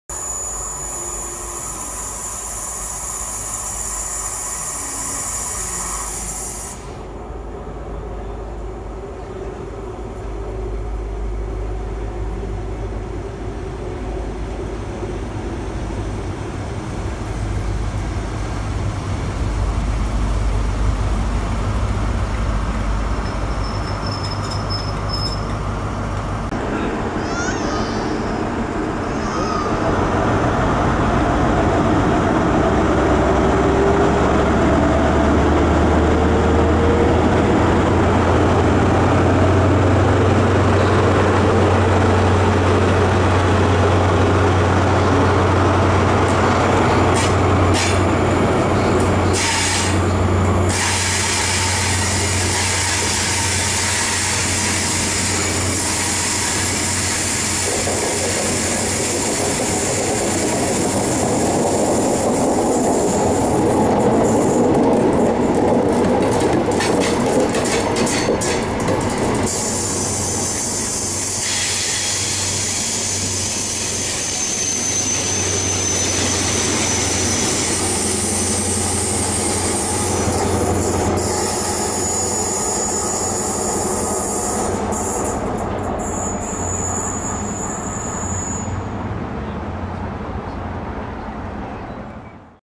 GWR 43027 90 Glorious Years + 43030 1C19 @ Bath Spa, 12/05/18
GWR 43027 90 Glorious Years + 43030 1C19 15:00 London Paddington - Bristol Temple Meads @ Bath Spa on the 12th May 2018.